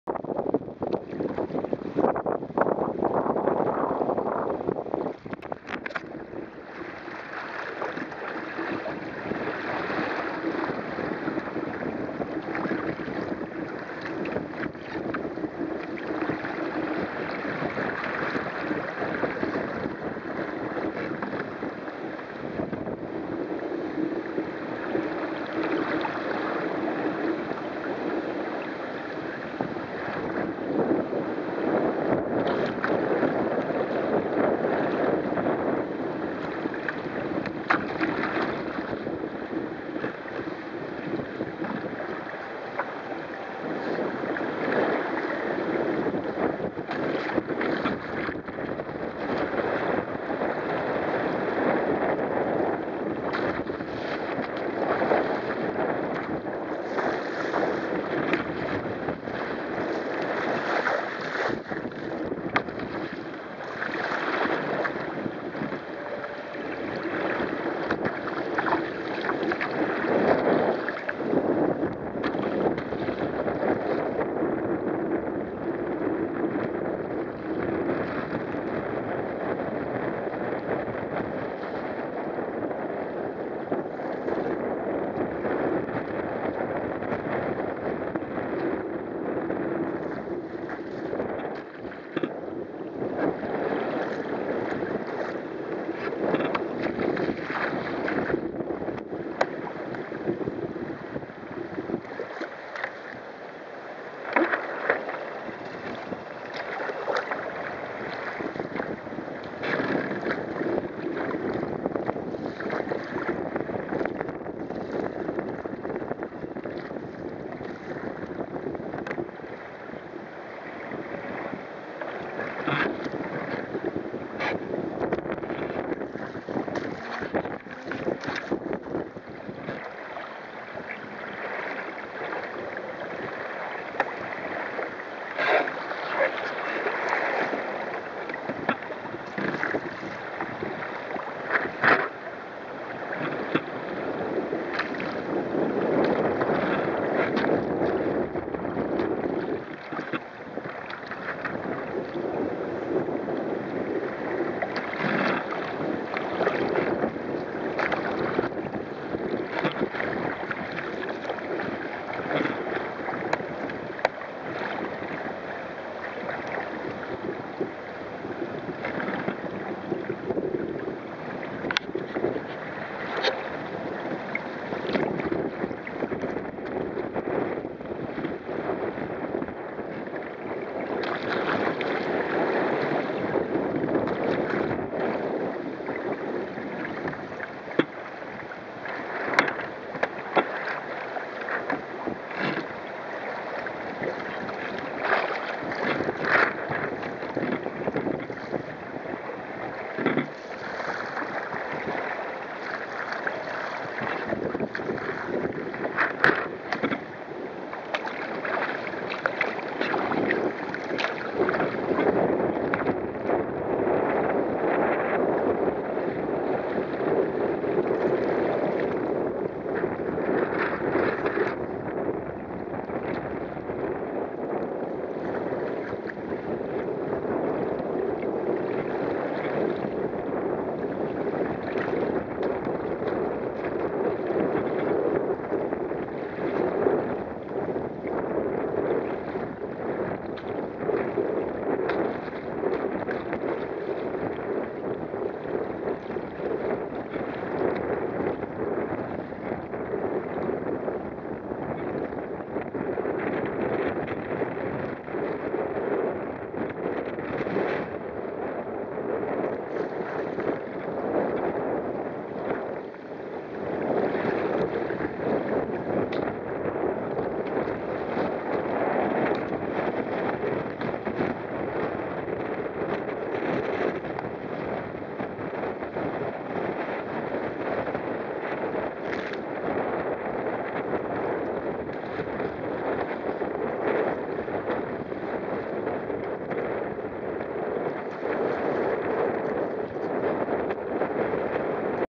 Kayak sailing on the maumee river with big wind and gusts.
Kayak sailing Maumee River September 30th 2011.

Strong wind.
Testing a new Falcon Sail design.

Some gybes and rendezvous.

P1000909.MOV